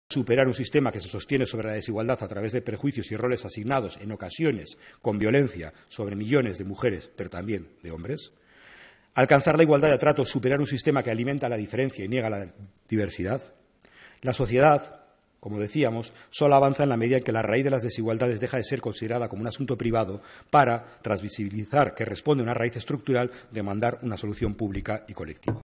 Igualdad entre mujeres y hombres: Un reto de país
Extracto de la ponencia de Igor Ahedo Gurrutxaga (UPV) en la presentación de la evaluación participativa de la Ley para la Igualdad.

¡Te esperamos!